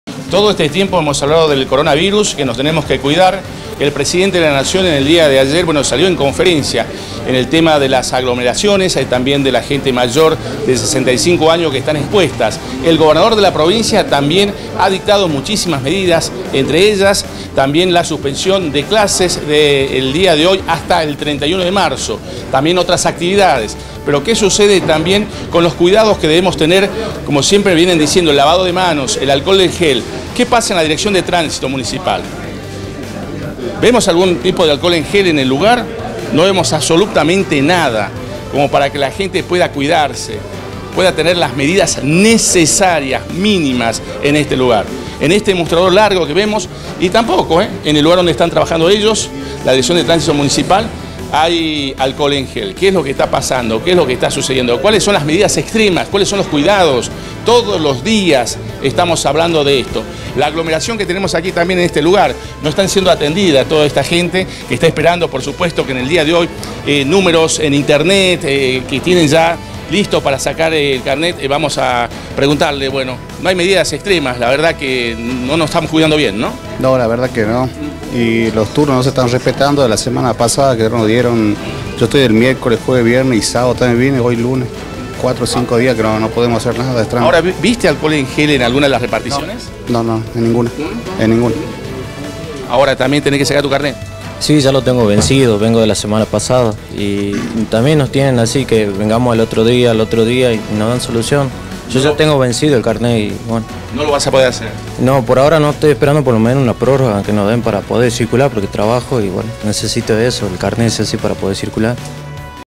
Tránsito municipal, ¿sin protocolo para el coronavirus?
Tránsito municipal, ¿sin protocolo para el coronavirus?. Contribuyentes molestos por demoras y falta de higiene